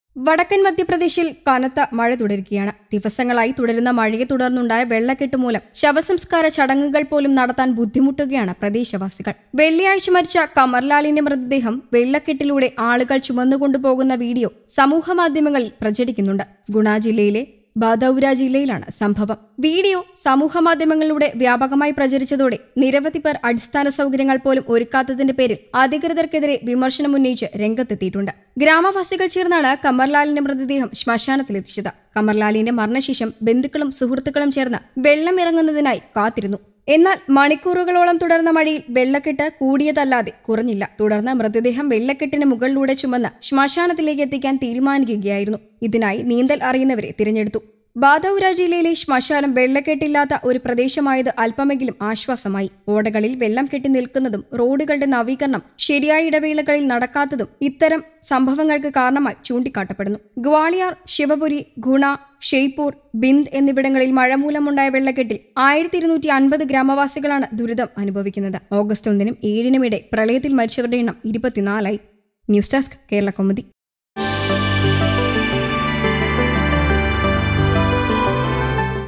Madhya Pradesh: Heavy rain lashes Khurai city, water level of rivers rise
വടക്കന്‍ മധ്യപ്രദേശില്‍ കനത്ത മഴ തുടരുകയാണ്. ദിവസങ്ങളായി തുടരുന്ന മഴയെത്തുടര്‍ന്നുണ്ടായ വെള്ളക്കെട്ടുമൂലം ശവസംസ്‌കാര ചടങ്ങുകള്‍പോലും നടത്താന്‍ ബുദ്ധിമുട്ടുകയാണ് പ്രദേശവാസികള്‍.

വെള്ളിയാഴ്ച മരിച്ച കമര്‍ലാലിന്റെ മൃതദേഹം വെള്ളക്കെട്ടിലൂടെ ആളുകള്‍ ചുമന്നുകൊണ്ട് പോകുന്ന വീഡിയോ സാമൂഹ്യ മാധ്യമങ്ങളില്‍ പ്രചരിക്കുന്നുണ്ട്. ഗുണ ജില്ലയിലെ ബാദൗരാ ജില്ലയിലാണ് സംഭവം. വീഡിയോ സാമൂഹ്യ മാധ്യമങ്ങളില്‍ വ്യാപകമായി പ്രചരിച്ചതോടെ നിരവധി പേര്‍ അടിസ്ഥാന സൗകര്യങ്ങള്‍ പോലും ഒരുക്കാതത്തിന്റെ പേരില്‍ ഏധികൃതര്‍ക്കെതിരെ വിമര്‍ശനം ഉന്നയിച്ച് രംഗത്തെത്തിയിട്ടുണ്ട്.

ഗ്രാമവാസികള്‍ ചേര്‍ന്നാണ് കമര്‍ലാലിന്റെ മൃതദേഹം ശ്മശാനത്തിലെത്തിച്ചത്. കമര്‍ലാലിന്റെ മരണശേഷം ബന്ധുക്കളും സുഹൃത്തുക്കളും ചേര്‍ന്ന് വെള്ളമിറങ്ങുന്നതിനായി കാത്തിരുന്നു. എന്നാല്‍, മണിക്കൂറുകളോളം തുടര്‍ന്ന മഴയില്‍ വെള്ളക്കെട്ട് കൂടിയതല്ലാതെ കുറഞ്ഞില്ല. തുടര്‍ന്ന് മ്യതദേഹം വെള്ളക്കെട്ടിന് മുകളിലൂടെ ചുമന്ന് ശ്മശാനത്തിലേക്ക് എത്തിക്കാന്‍ തീരുമാനിക്കുകയായിരുന്നു. ഇതിനായി നീന്തല്‍ അറിയുന്നവരെ തിരഞ്ഞെടുത്തു. ബാദൗരാ ജില്ലയിലെ ശ്മശാനം  വെള്ളക്കെട്ട് ഇല്ലാത്ത ഒരു പ്രദേശമായത് അല്‍പ്പമെങ്കിലും ആശ്വാസമായി. ഓടകളില്‍ വെള്ളംകെട്ടി നില്‍ക്കുന്നതും റോഡുകളുടെ നവീകരണം ശരിയായ ഇടവേളകളില്‍ നടത്താത്തതും ഇത്തരം സംഭവങ്ങള്‍ക്ക് കാരണമായി ചൂണ്ടിക്കാട്ടപ്പെടുന്നു.

ഗ്വാളിയാര്‍, ശിവപുരി, ഗുണ, ഷെയ്പൂര്‍, ബിന്ദ് എന്നിവിടങ്ങളില്‍ മഴമൂലമുണ്ടായ വെള്ളക്കെട്ടില്‍ 1250 ഗ്രാമവാസികളാണ് ദുരിതം അനുഭവിക്കുന്നത്. ഓഗസ്റ്റ് ഒന്നിനും ഏഴിനും ഇടെ പ്രളയത്തില്‍ മരിച്ചവരുടെ എണ്ണം 24 ആയി.